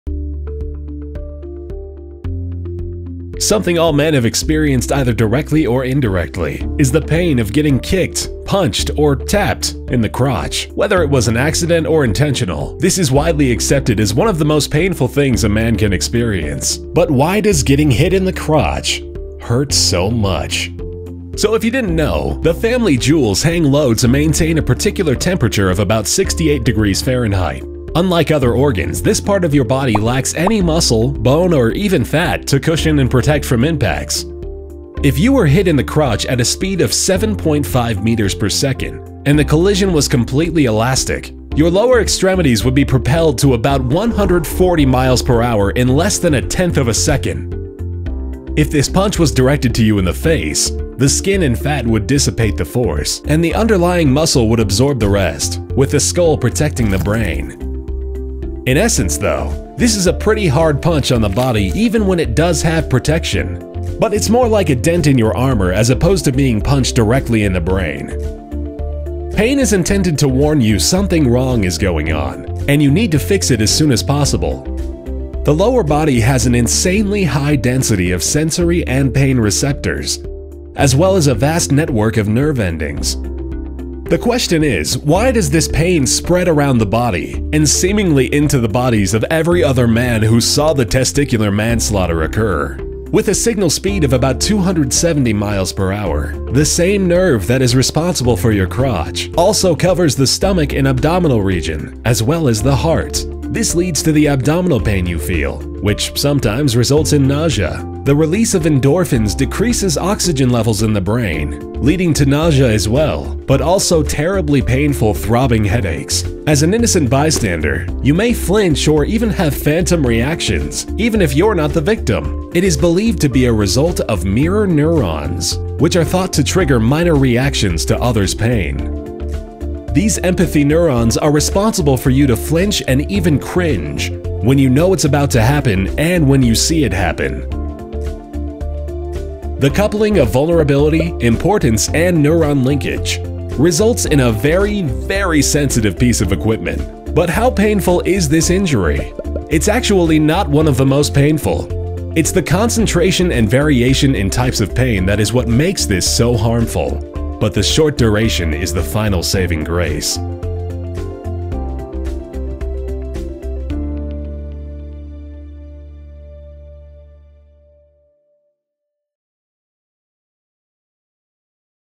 Getting Hit In The Balls (Scientific Analysis)
We take an in-depth look at what's actually happening when you get hit in family jewels, and why it hurts so much!